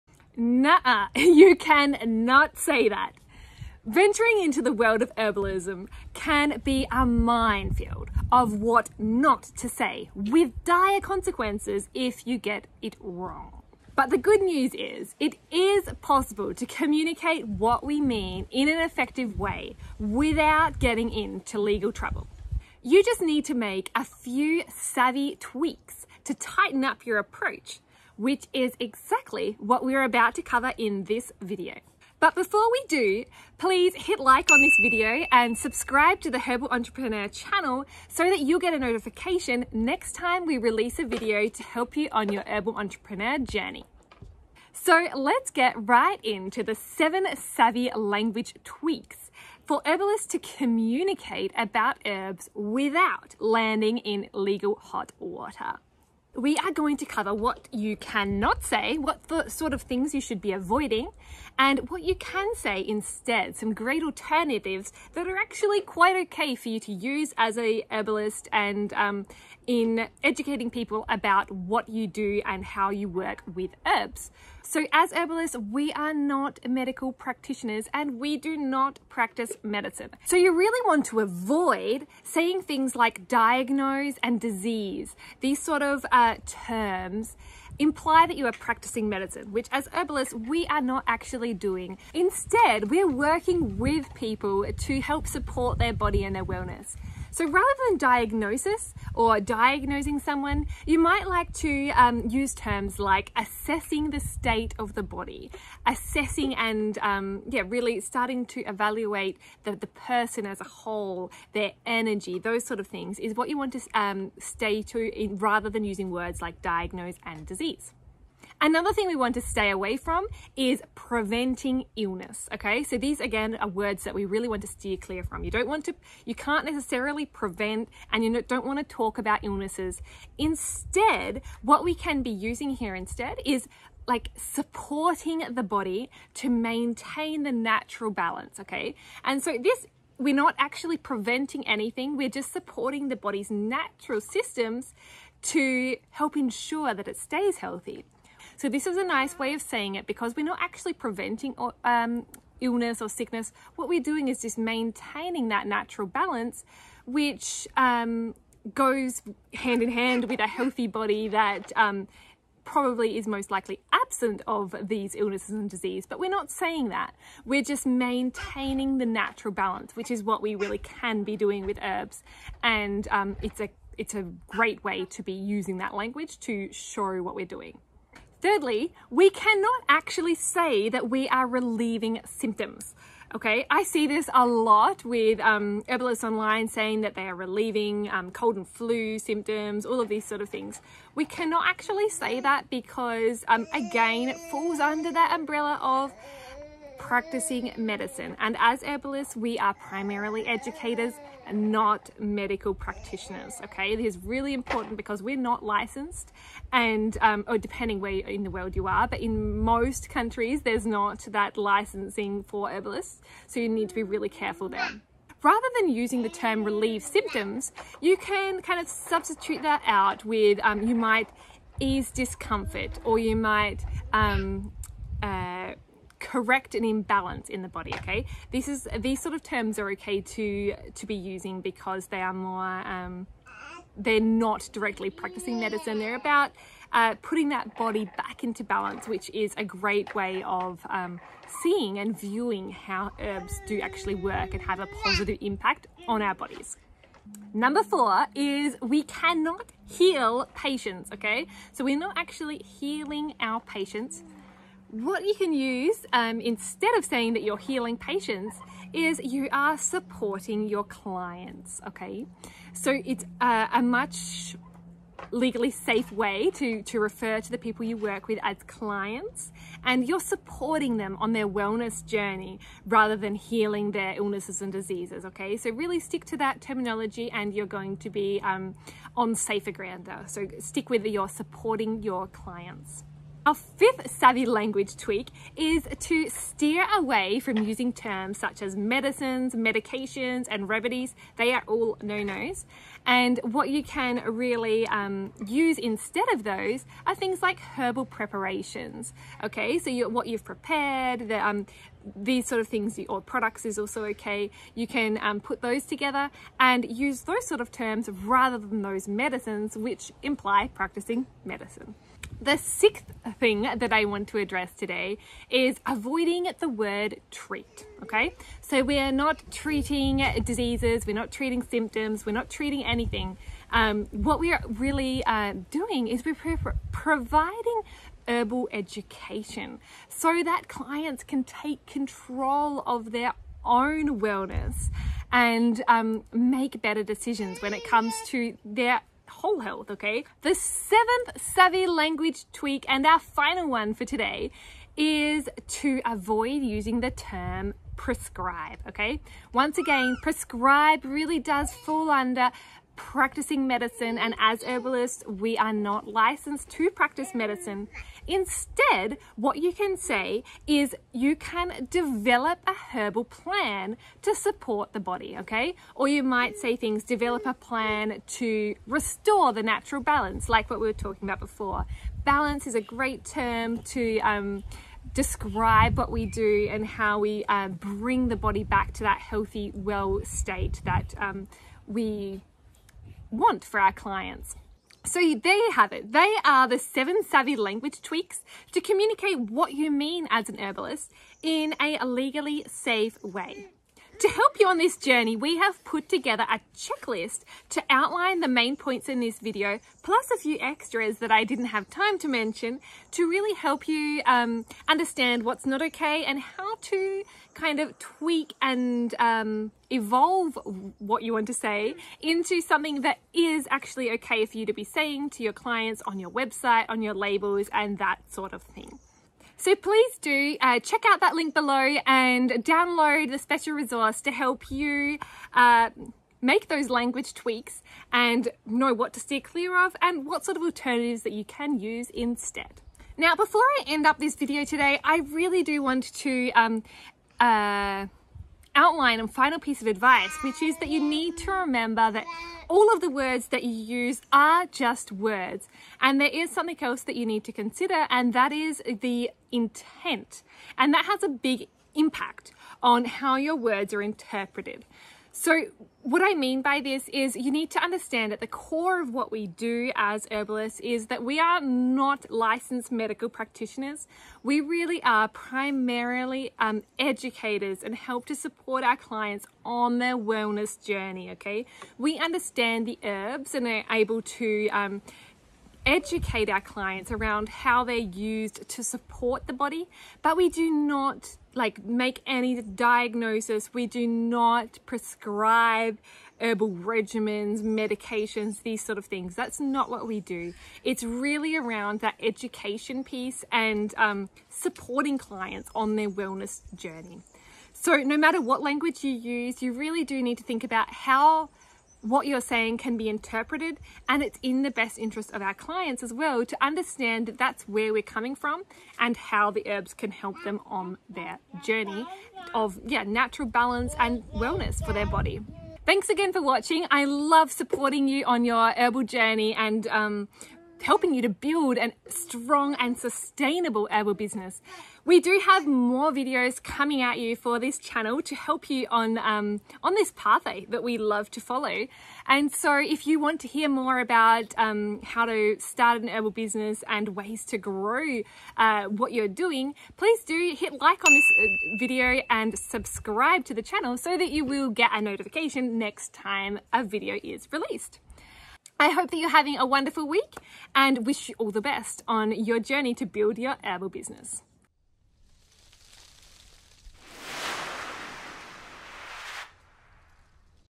7 Savvy Language Tweaks For Herbalists To Communicate About Herbs Without Landing In Legal Hot Water
Venturing into the world of herbalism can be a minefield of what not to say with dire consequences if you get it wrong.

But the good news is, it's possible to communicate what we mean in an effective way without getting into legal trouble.

You just need to make a few savvy tweaks to tighten up your approach, which is exactly what we're about to cover.